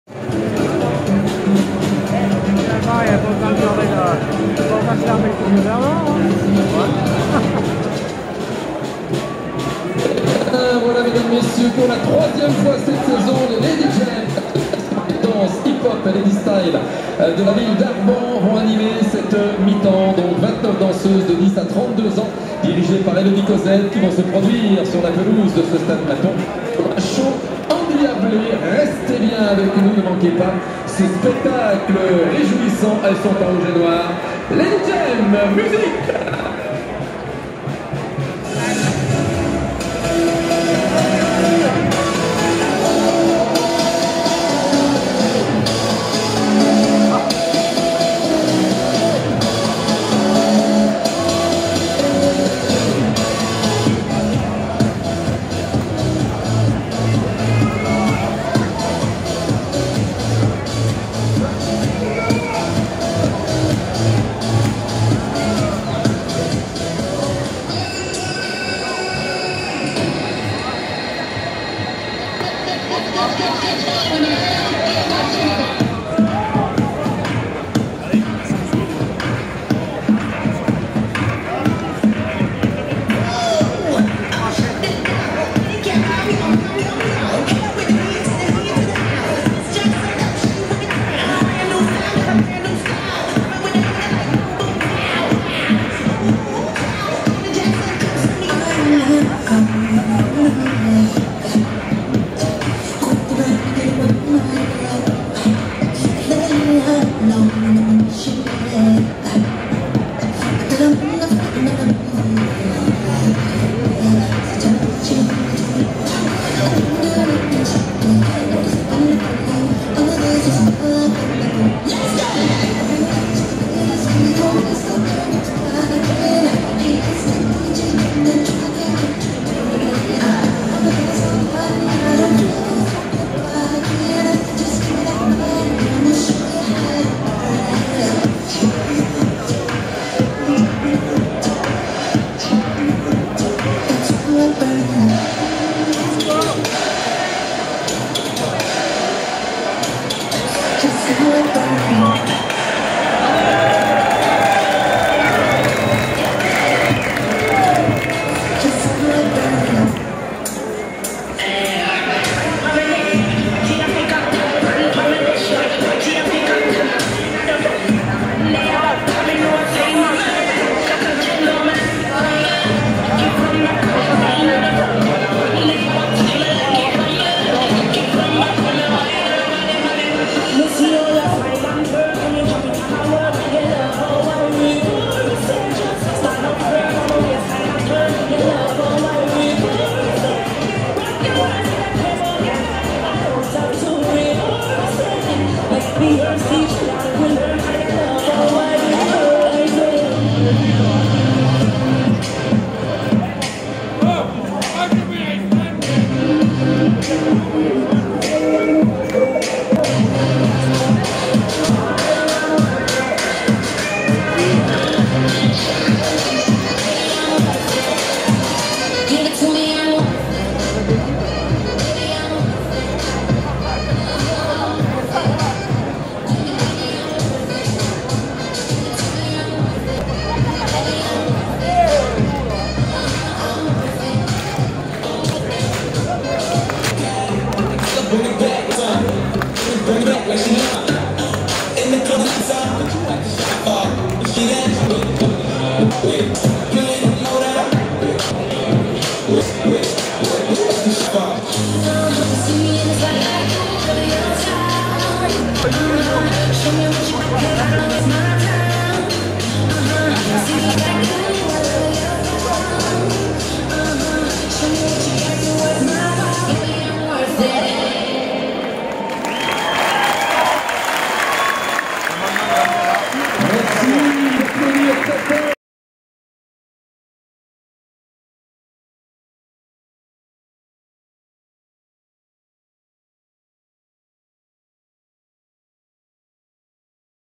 Show Pom Pom Girl Lady'jam - Mi-Temps - Oyonnax / LOU (Lyon) - Top 14 - HD / 4K (28/04/18)
Un jolie spectacle/show de Pom pom Girl Lady'jam à la mi-temps du match Oyonnax / L.O.U (Lyon) qui avait lieu le Samedi 28 Avril 2018 vers les 21h35 environs que c'était une représentation au Stade Charles Mathon à Oyonnax dans l'Ain (01) !

Voici leurs page facebook Lady'jam que je vous inviter à aimé leurs